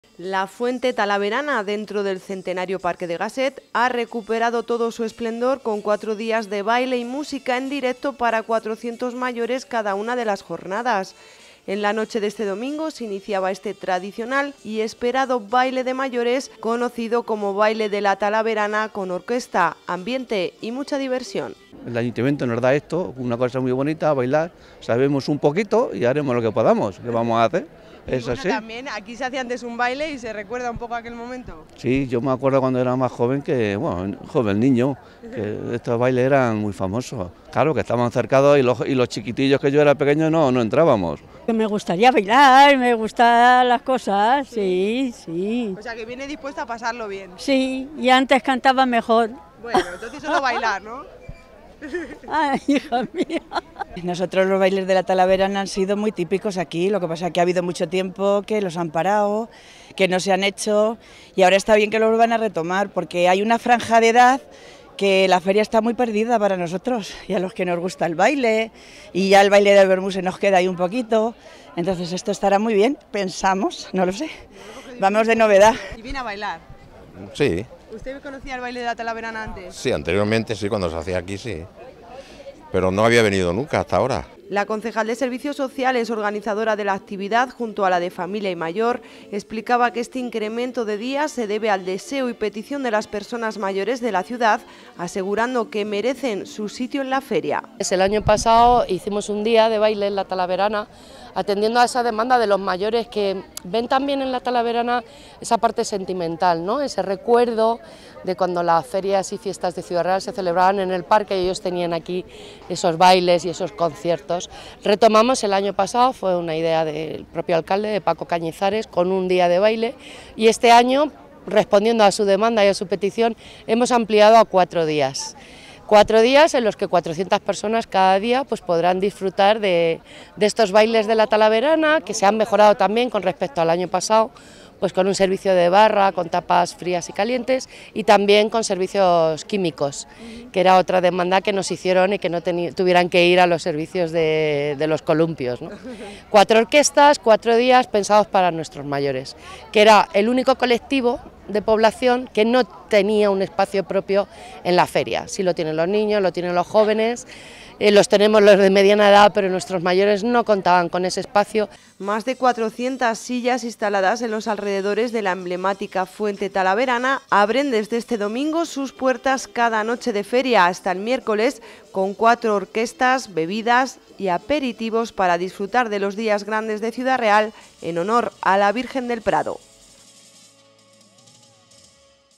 La Fuente Talaverana se abre a 4 días de baile y orquesta con más de 400 mayores cada día
La Fuente Talaverana, dentro del centenario Parque de Gasset, ha recuperado todo su esplendor con cuatro días de baile y música en directo para 400 mayores cada una de las jornadas. En la noche de este domingo se iniciaba este tradicional y esperado baile de mayores, conocido como “baile de La Talaverana”, con orquesta, ambiente y mucha diversión.